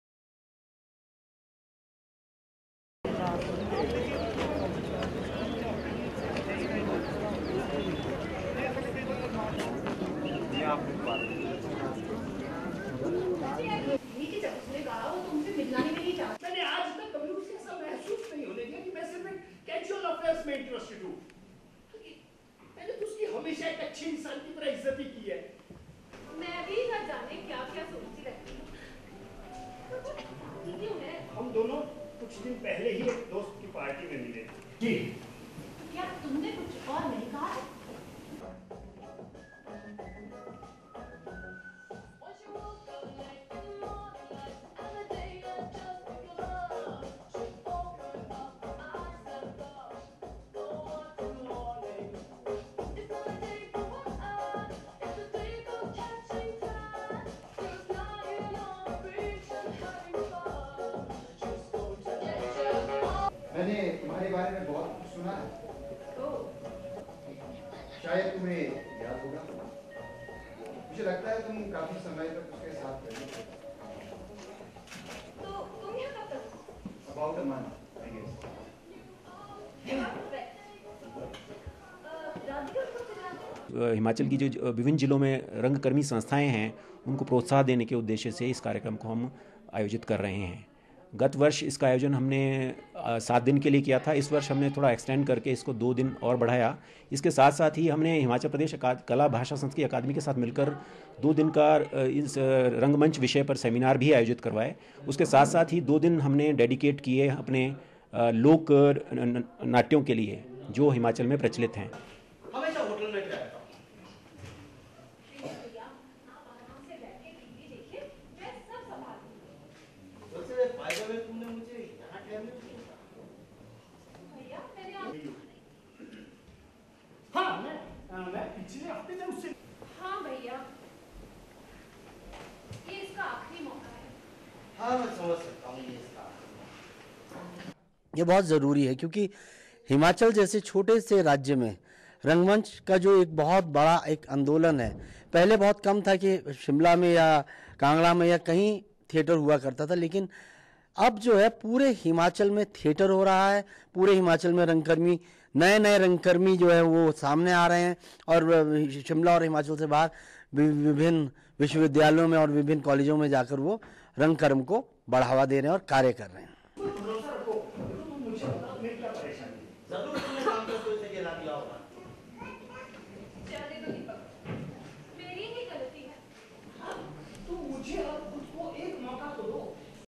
28,Mar 2015 - Theatre festival promoting new talent ends in India's northern hill town
28,Mar 2015 (ANI) - A nine-day theatre festival aiming to promote arts, culture and young talent ends in India's northern hill town Shimla.